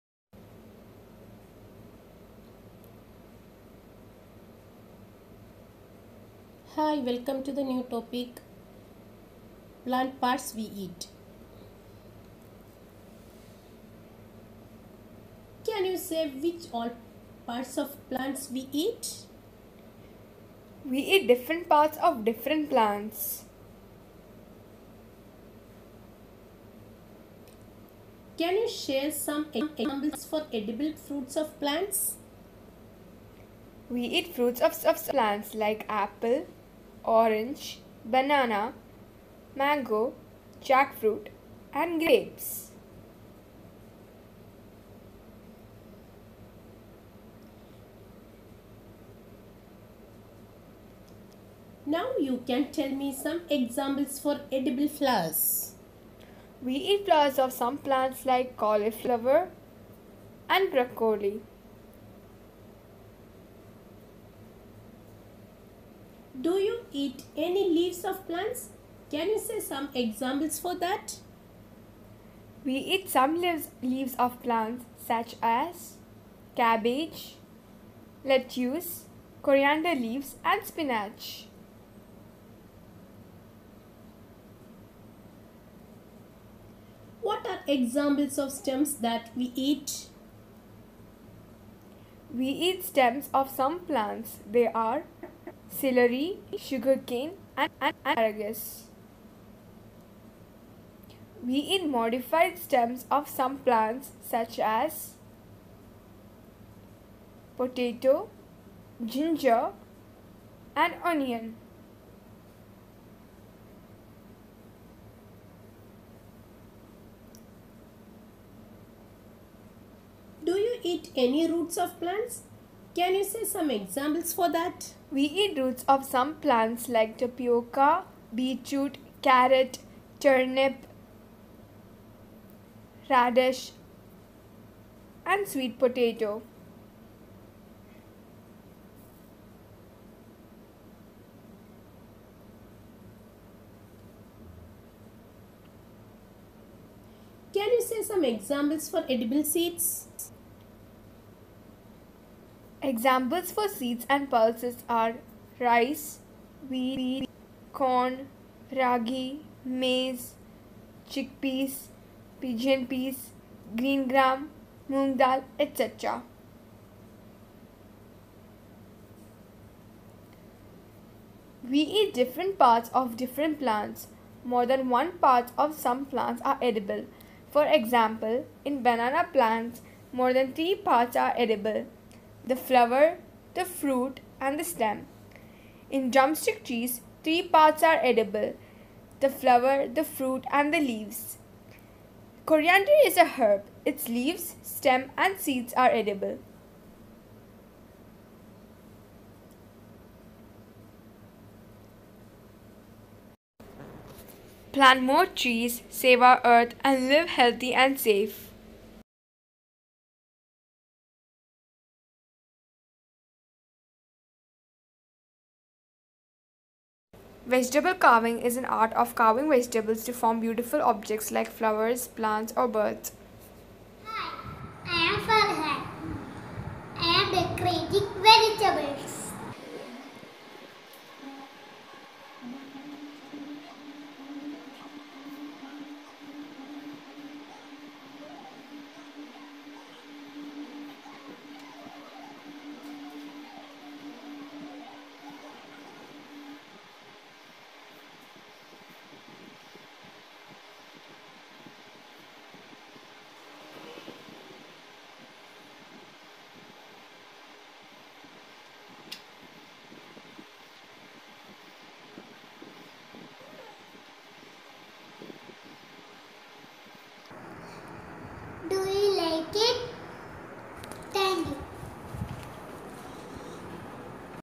Plant Parts We Eat - Stems, Roots, Flowers, Fruits, Leaves and more! Grade 2
Find out about edible parts of plants. Learn and Grow with Abi.